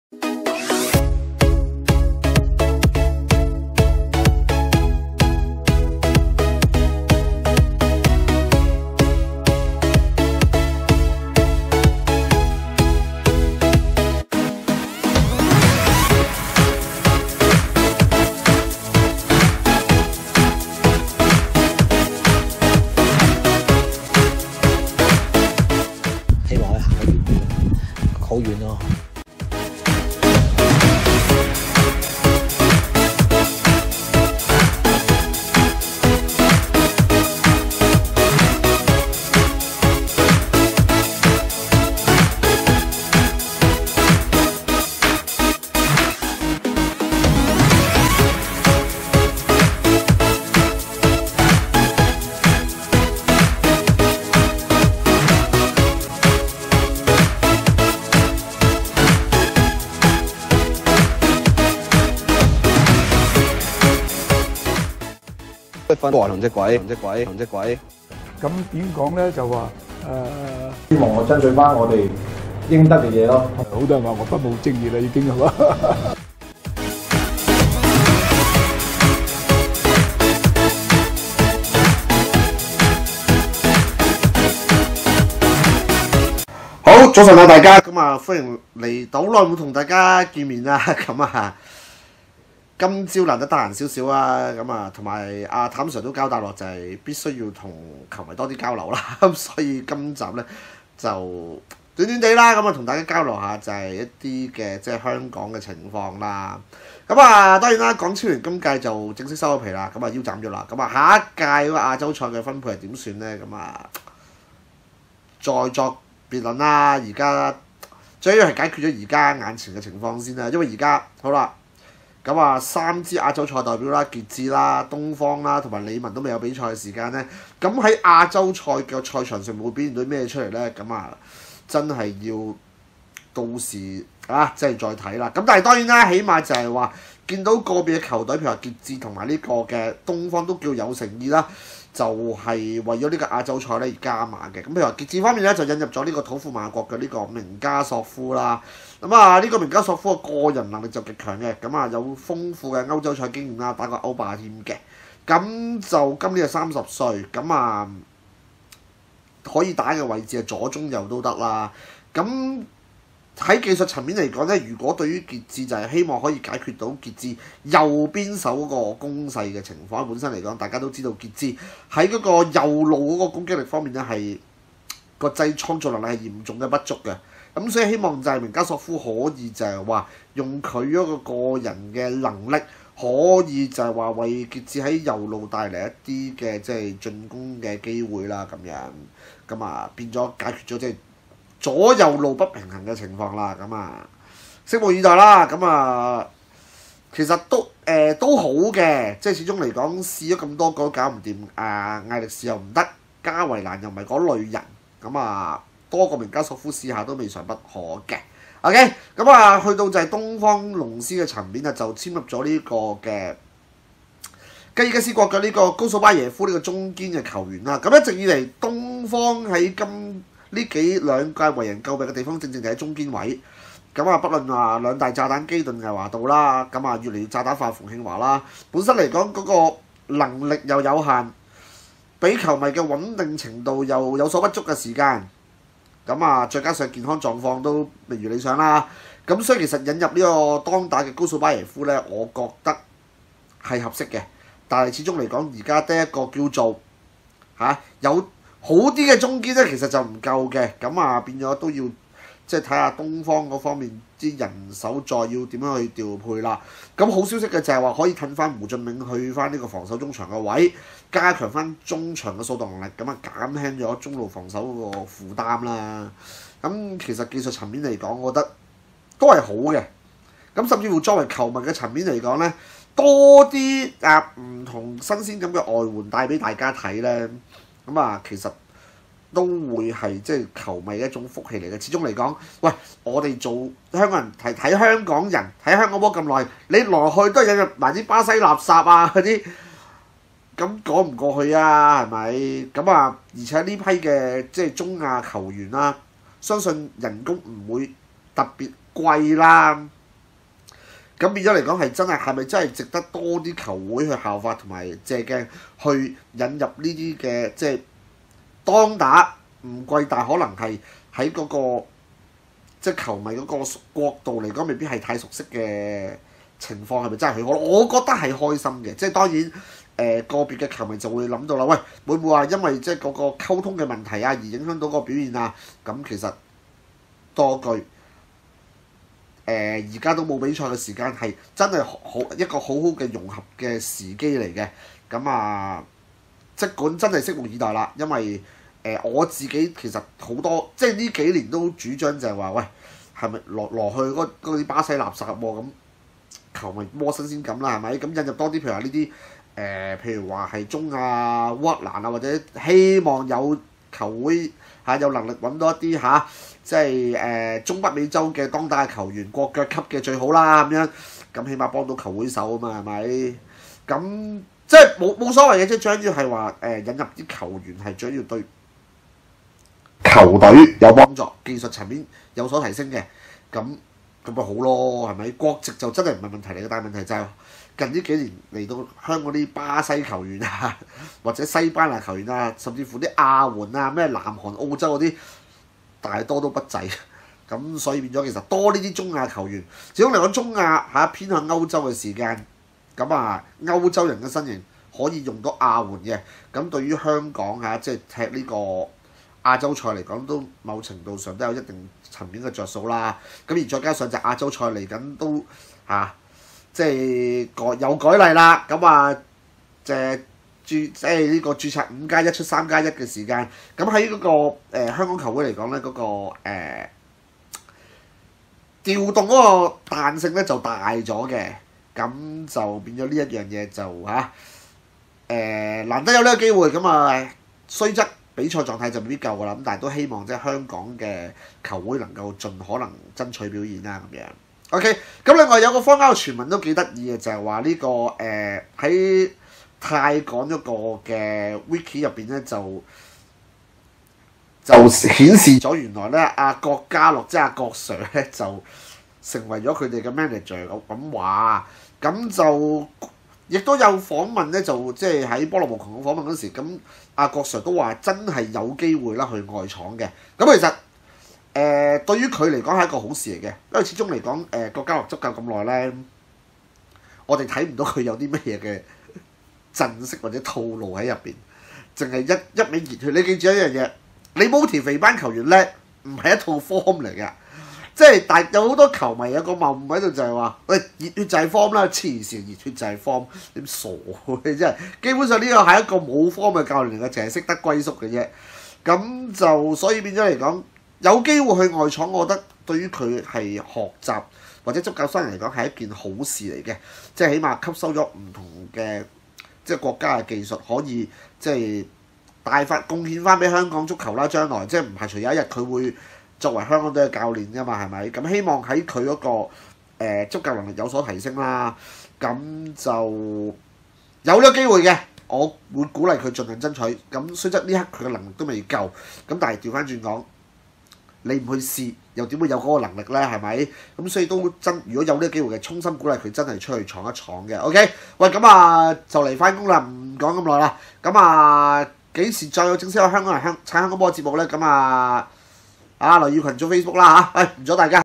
香港人撐香港波 第203集 20220310 雜談中亞新兵！鼓勵郭嘉諾：有得外闖必須去！
MC:Mike

【球迷直播室】
網絡節目平台，用心制作，請多多支持！

Follow Instagram 帳號： football_live_hk